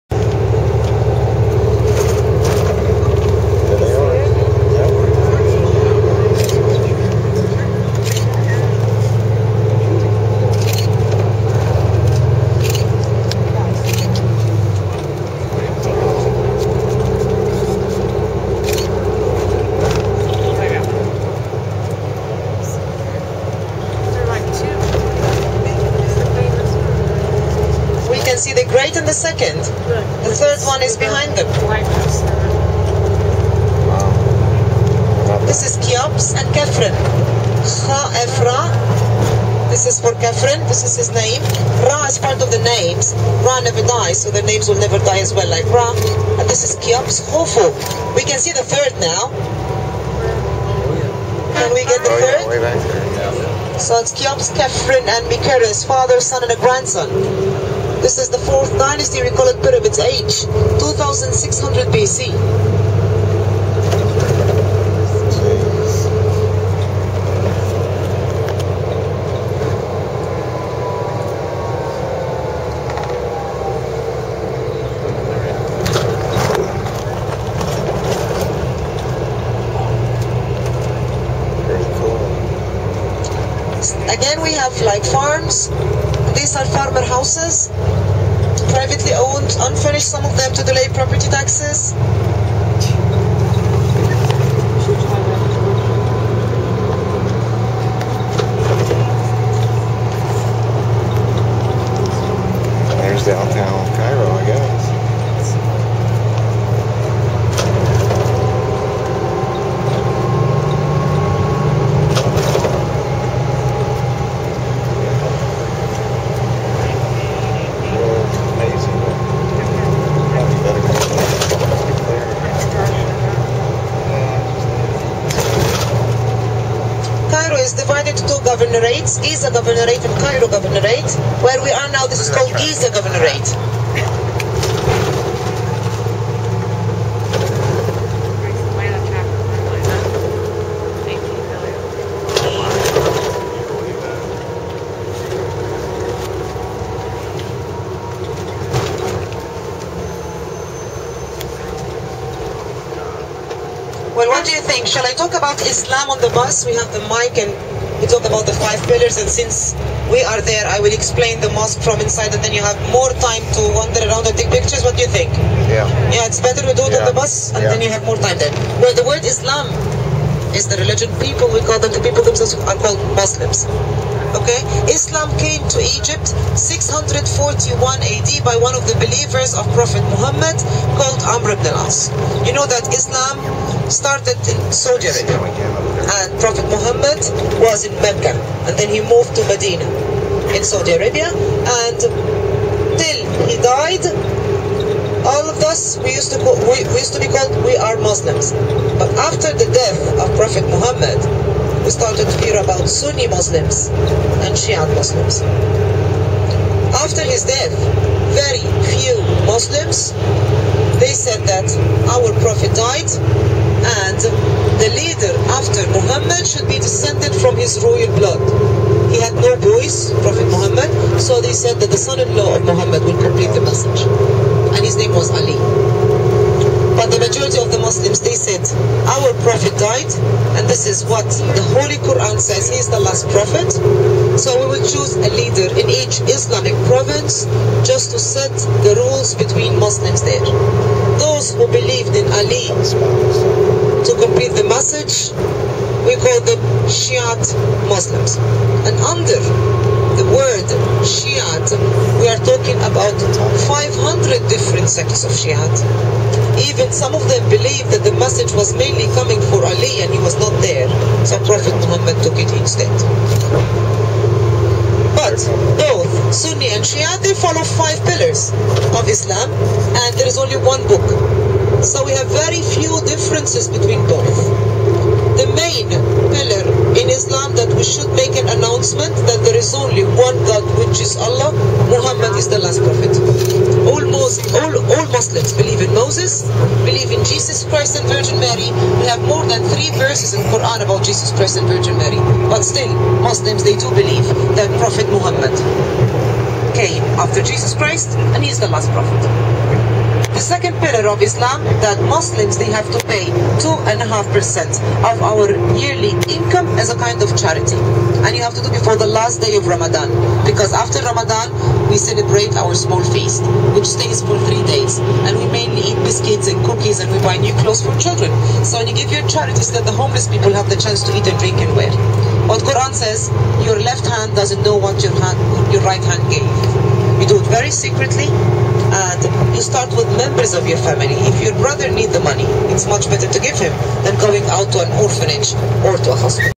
Explanation of Islam-Sunni and Shiite differences
During our trip to Cairo Egypt our tour guide explains Islam, Sunni and Shiite differences, what the similarities are, beliefs of Muslims in Moses and Jesus and more. Very informative!